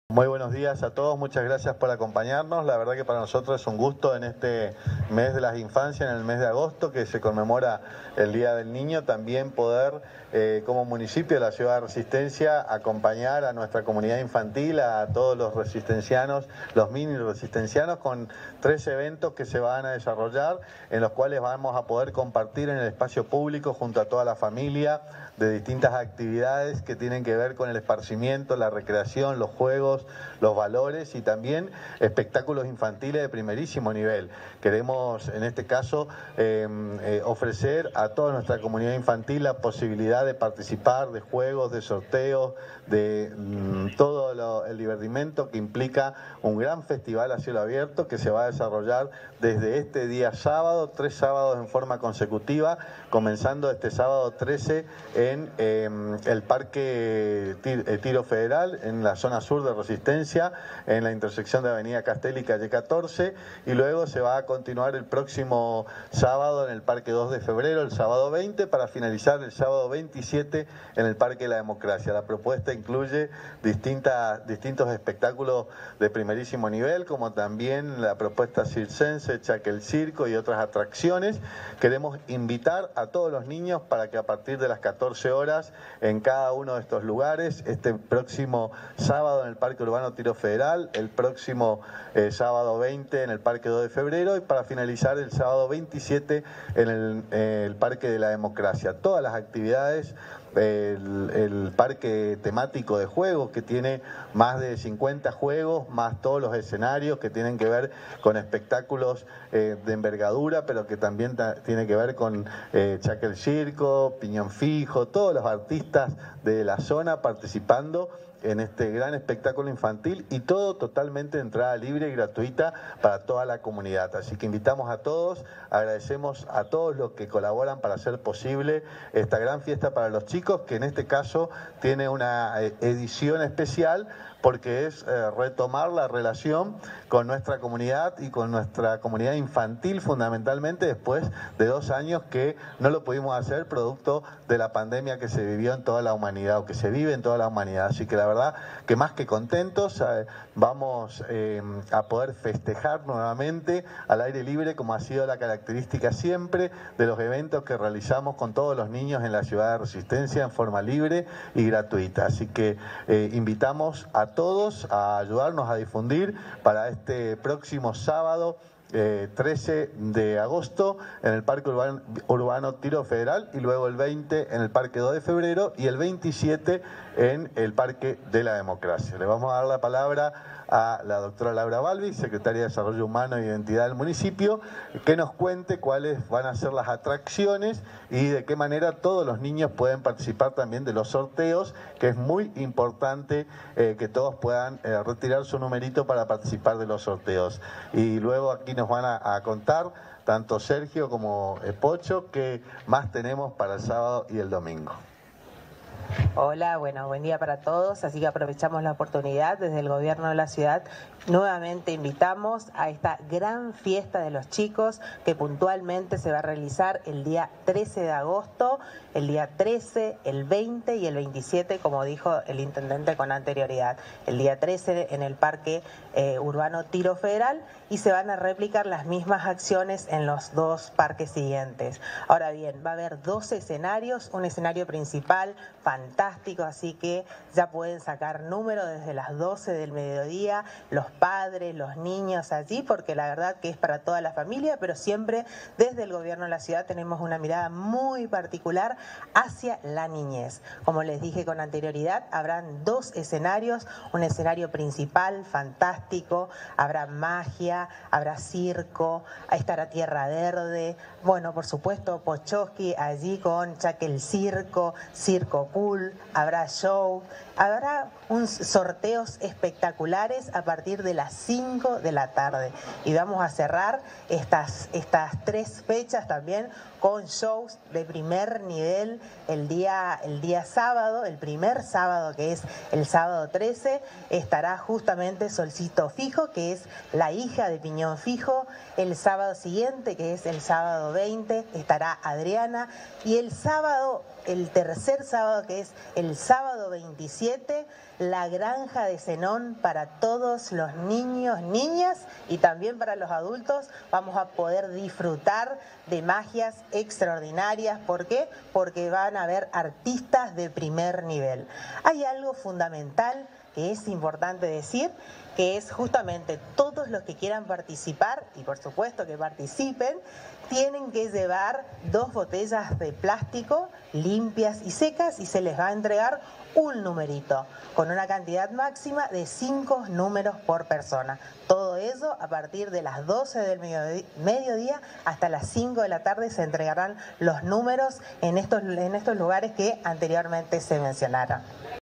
PRESENTAN LA GRAN FIESTA DE LOS NIÑOS
ORGANIZADO POR EL MUNICIPIO
GUSTAVO MARTÍNEZ INTENDENTE
LAURA BALBIS SECRETARIA DE DESARROLLO HUMANO E IDENTIDAD